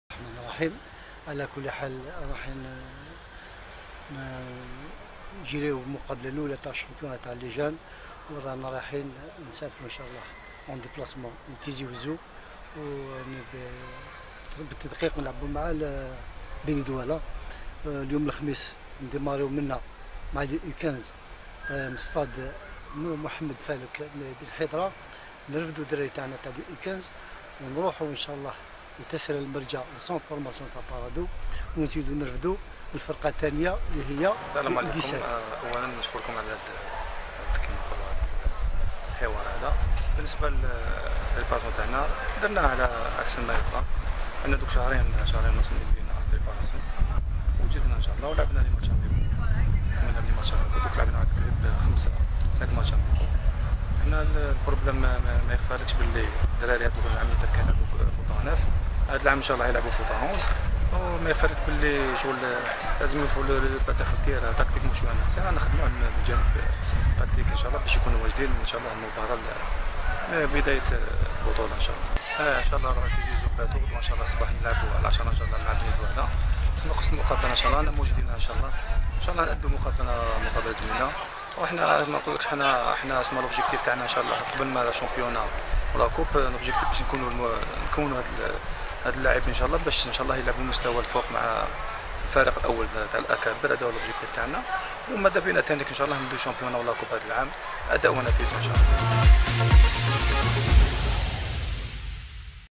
interview avant match: USBD vs PAC
US Beni Douala vs Paradou AC (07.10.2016) U15 & U17
Premier match officiel de la saison 16/17
interview avant match